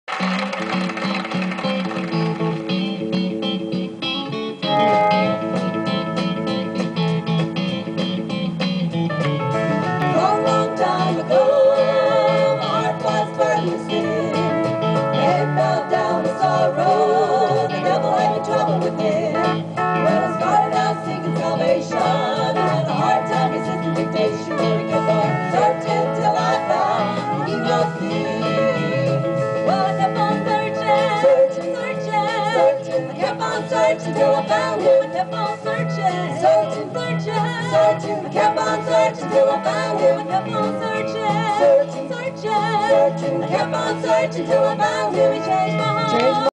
New Journey Quartet Live 2008
This is a short video clip of "The New Journey Quartet" performing live in 2008 on the Dove Broadcast Network.